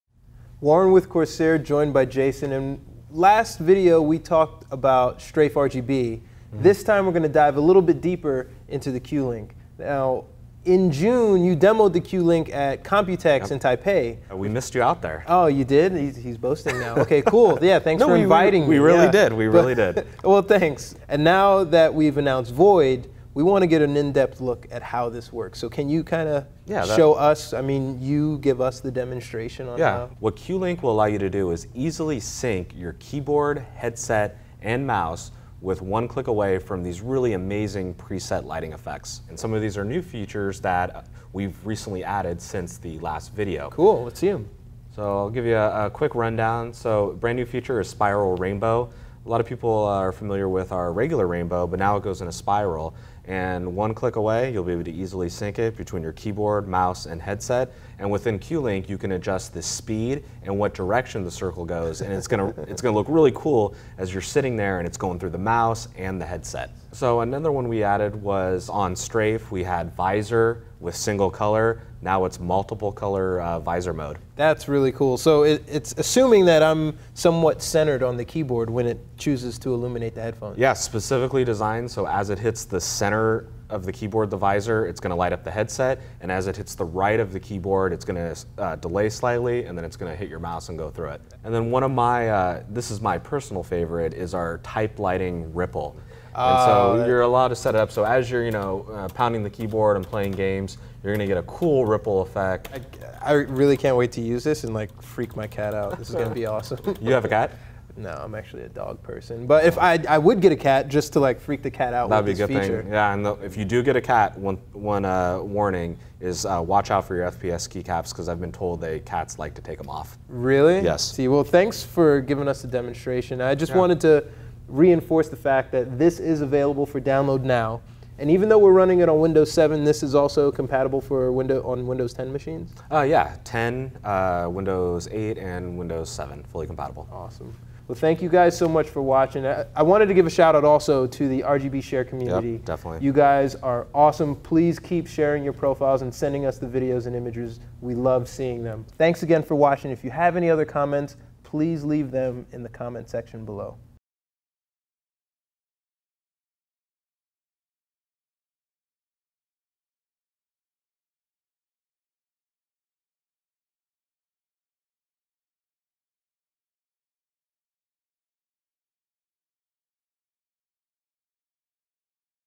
Corsair CUE Link RGB software demo
Now you can synchronize lighting between your Corsair RGB mouse, keyboard, and headset -- anything from simple color coordination, to sophisticated animation sequences that ripple and bounce across your desktop.